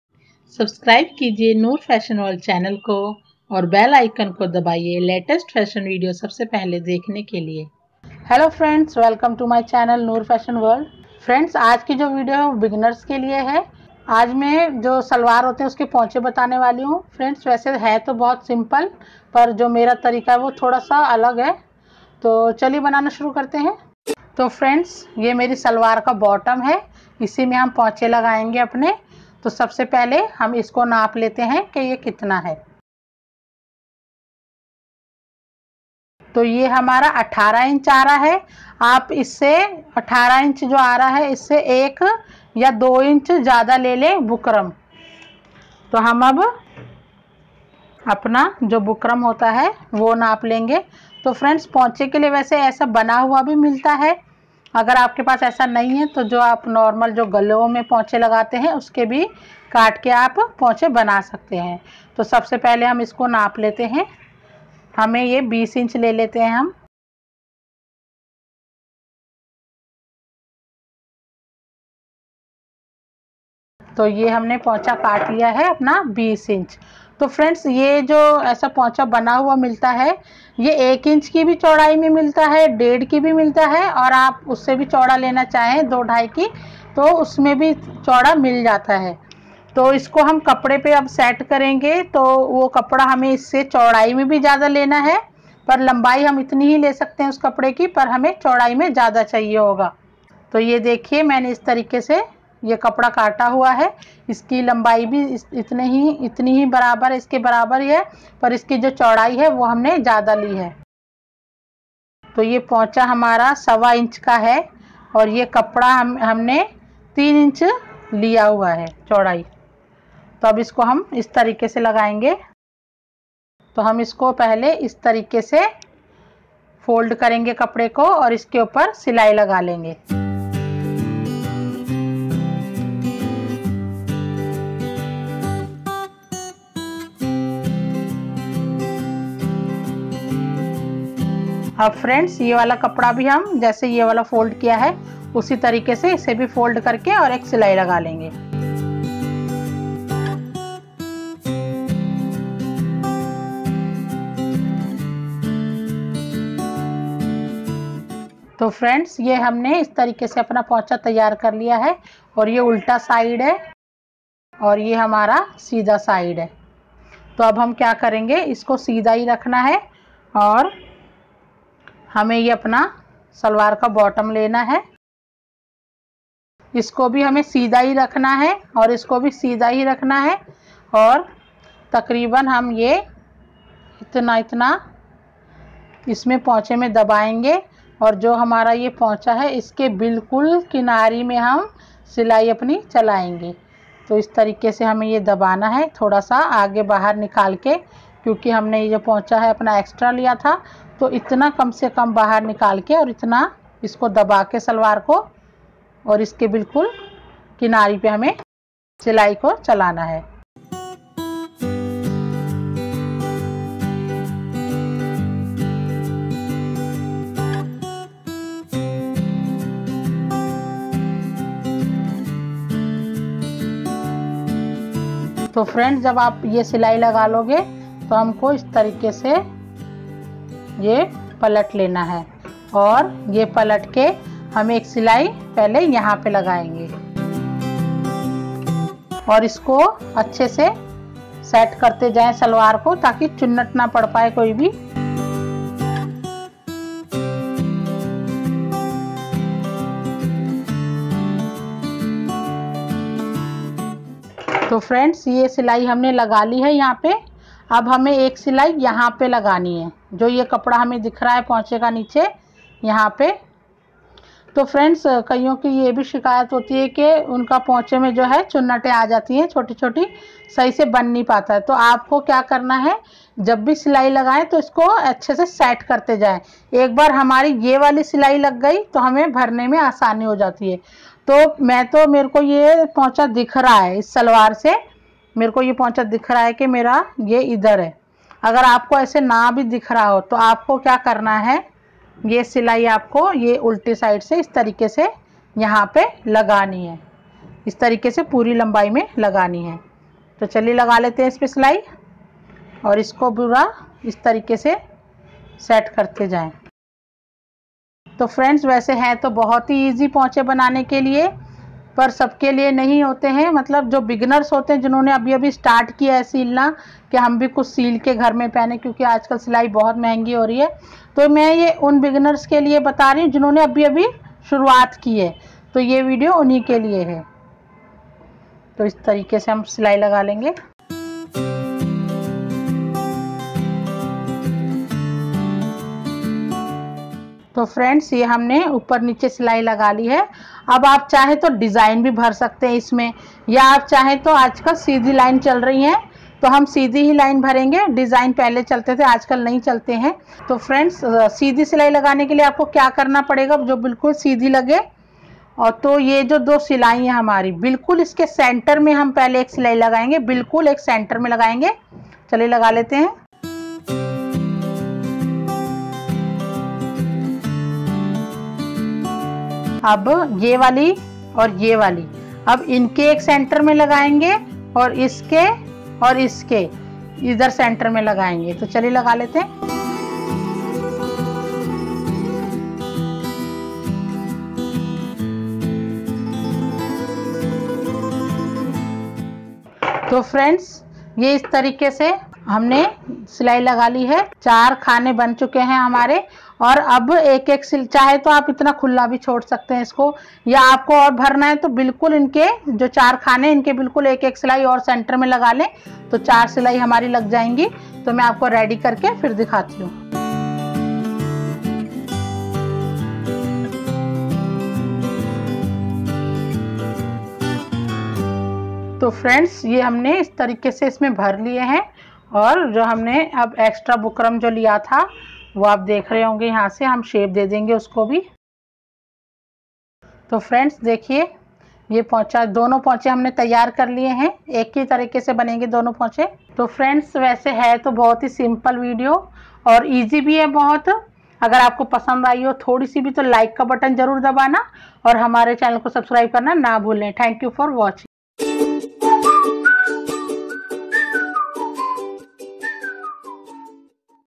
How to attach Poncha in salwar perfectly | Salwar bottom making for beginners
How to attach poncha in salwar. Salwar bottom.
In this video i will show you how to cut and stitch poncha of salwar and will give you some very useful tips and ideas for attaching ponche in salwar.
In this video i will give you very helpful tips for salwar ponche. This video have full tutorial of Poncha cutting and stitching in professional style.

Hi Friends, Welcome to Noor fashion world.

Also watch.